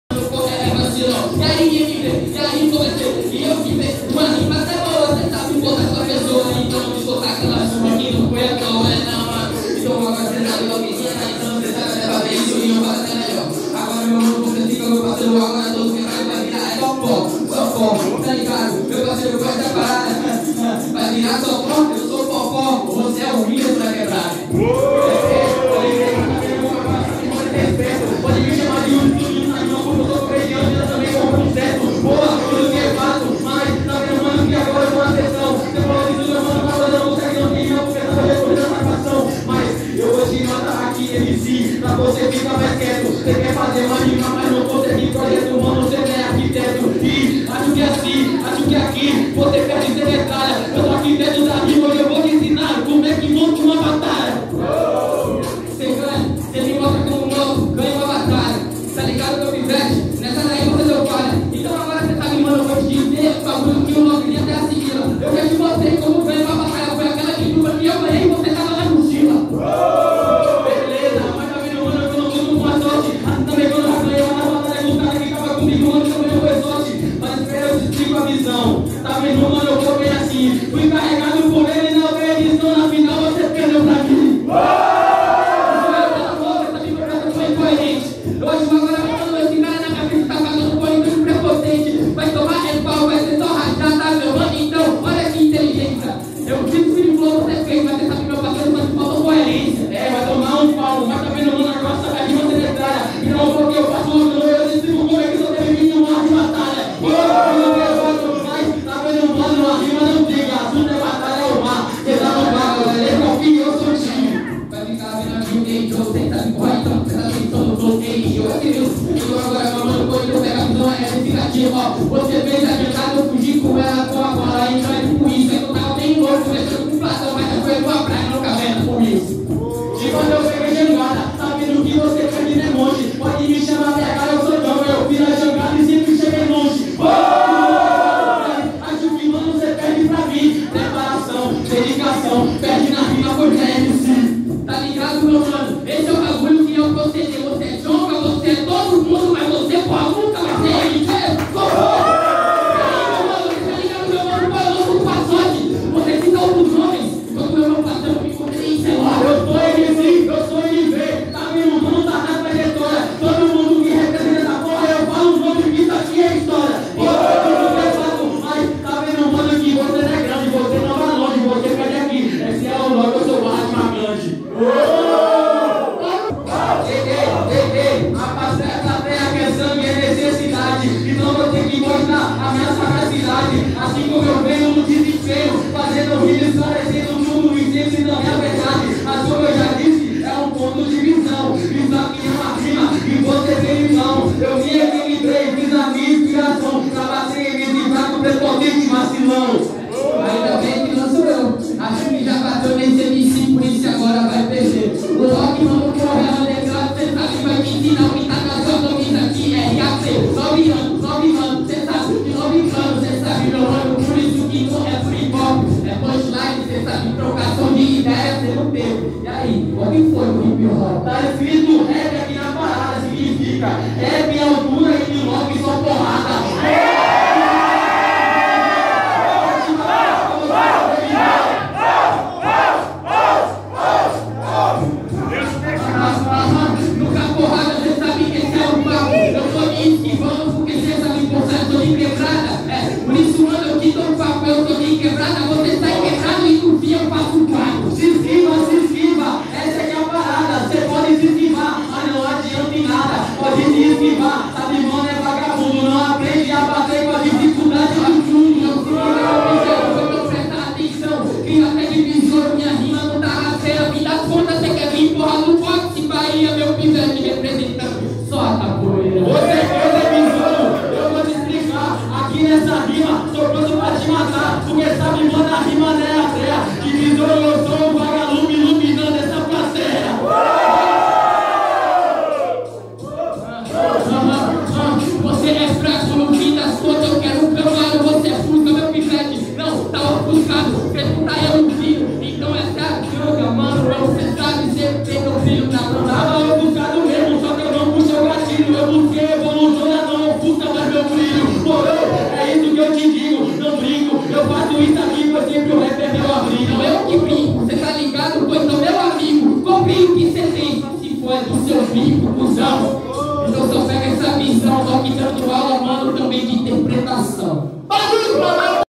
(PEGOU FOGO 🔥🔥) MV X LOKI - LAB GRIOT PORTO SEGURO - 175° EDIÇÃO - BA
A Batalha do complexo é um movimento cultural da cidade de Porto Seguro -Ba que nessa edição fez parceria com o circuito de rimas improvisada, @3°round!
Apoio: Kalifa, @Bit_Autentic

BATALHA TODO DOMINGO NA PRAÇA DO TRABALHADOR - BAIANÃO (PORTO SEGURO-BA)